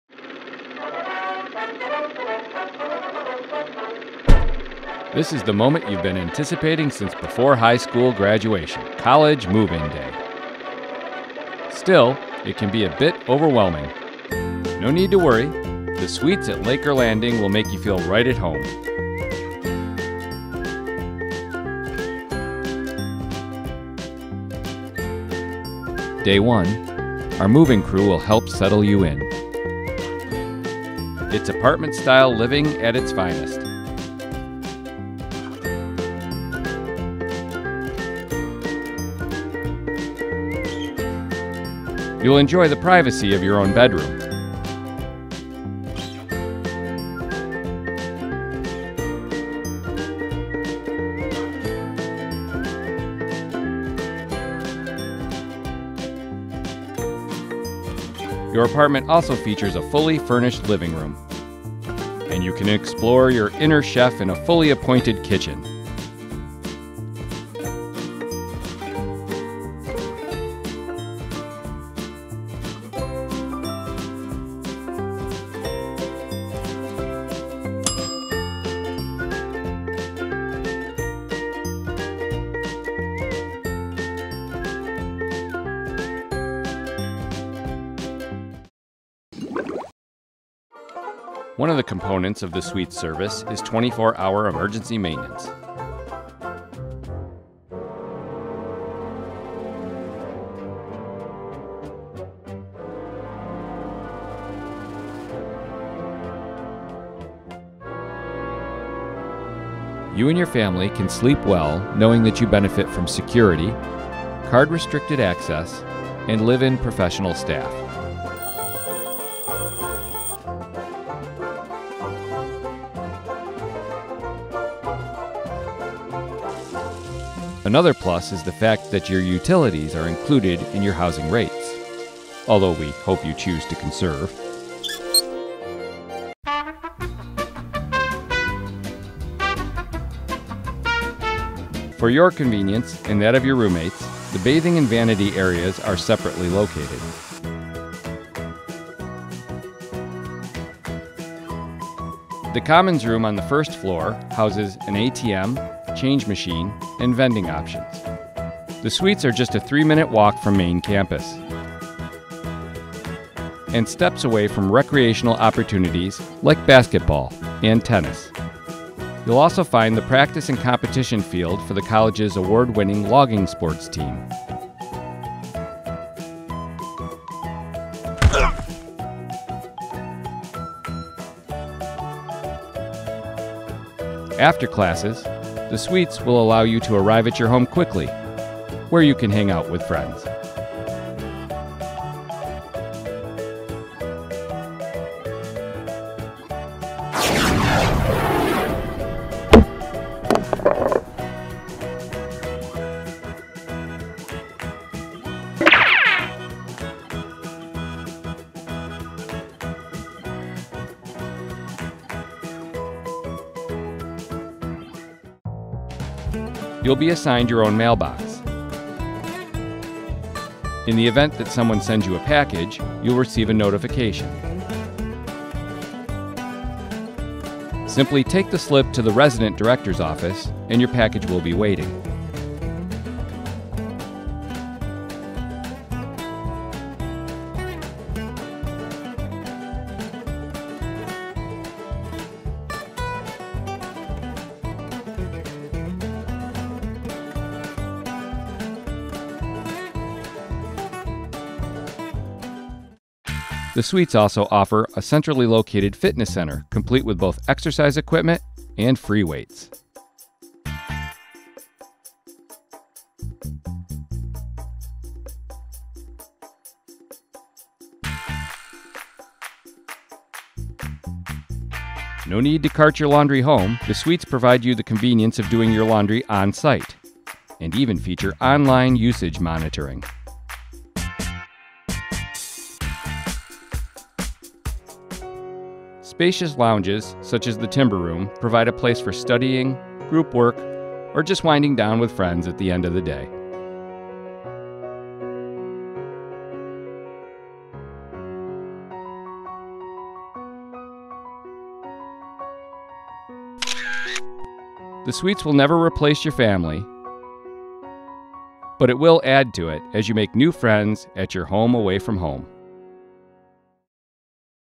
Flick's Suite Life
Take a tour of the Suites at Laker Landing courtesy of Finger Lakes Community College's mascot, Flick.